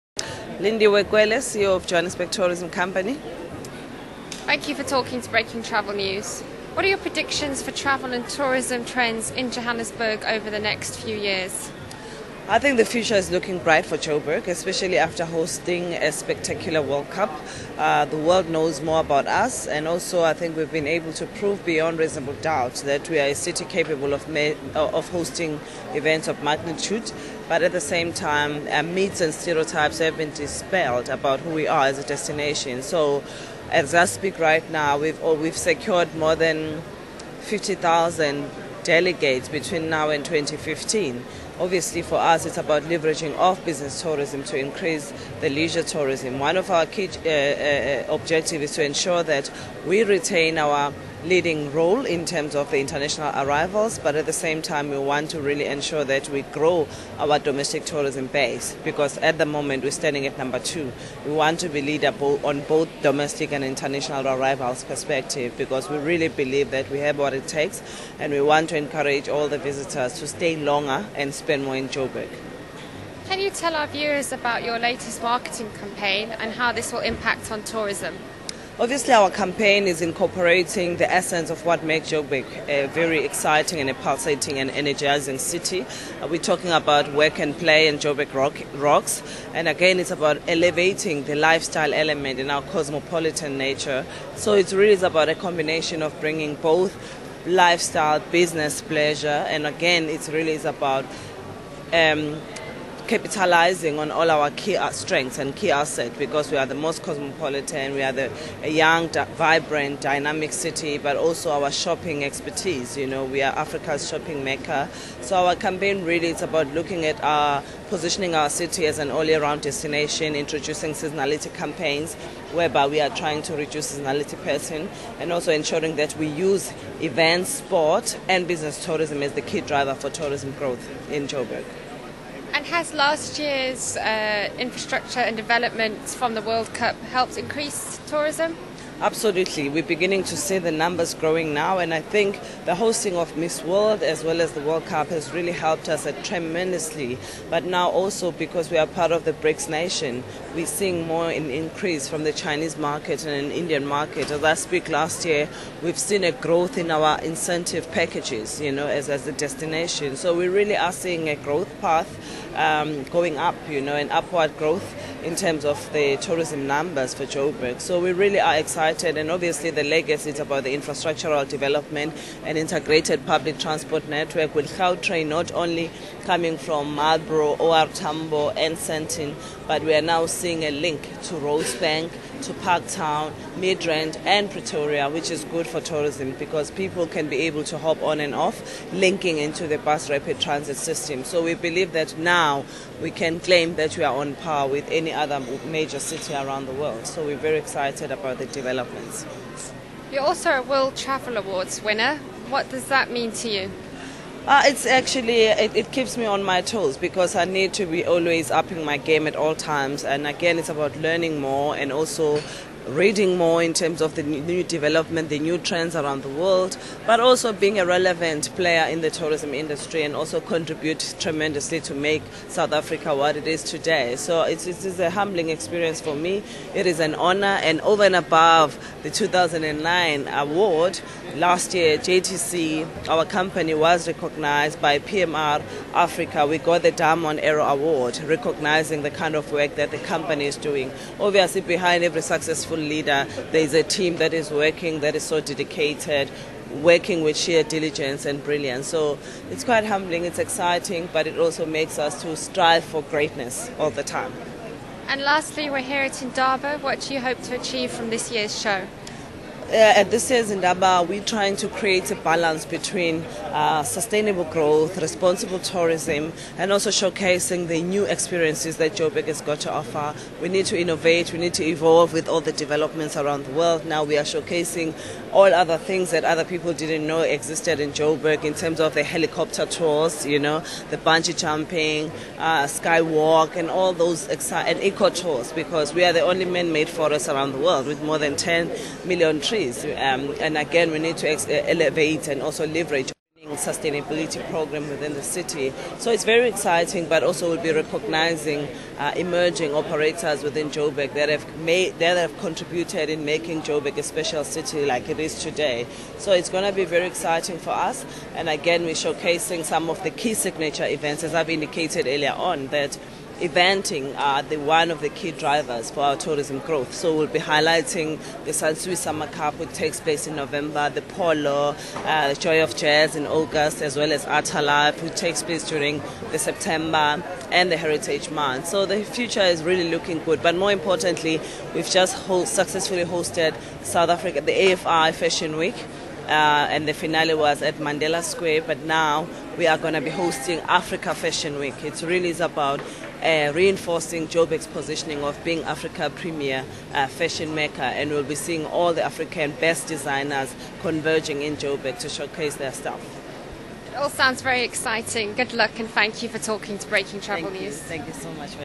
Lindiwe Kwele, CEO of Johannesburg Tourism
Lindiwe Kwele, CEO of Johannesburg Tourism  talks to Breaking Travel News at Indaba 2011